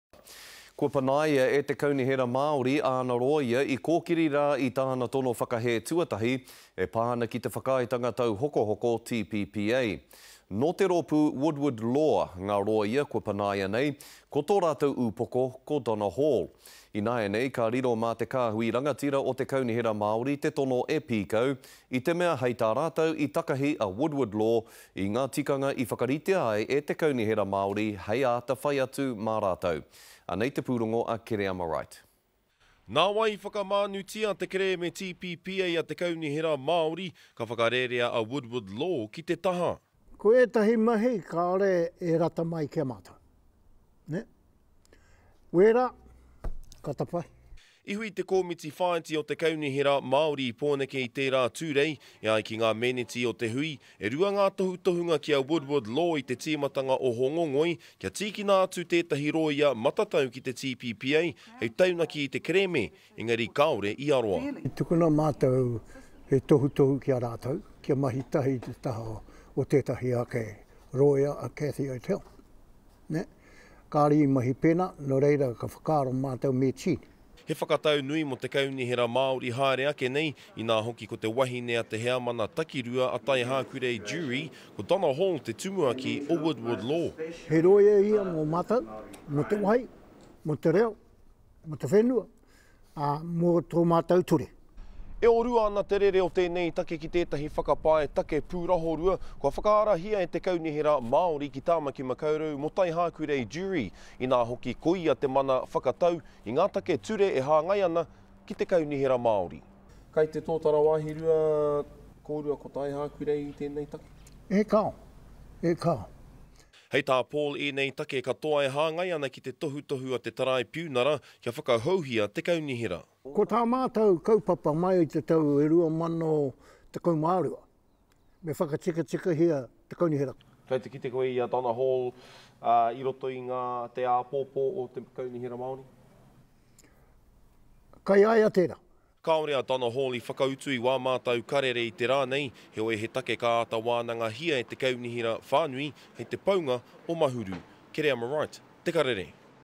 NZ Māori Council dismisses long-time legal counsel
The NZ Māori Council has dismissed its original counsel for its TPPA claim. Woodward Law, who is represented by Donna Hall, has been replaced by the executive of the Māori Council, after breaching directives given by the executive.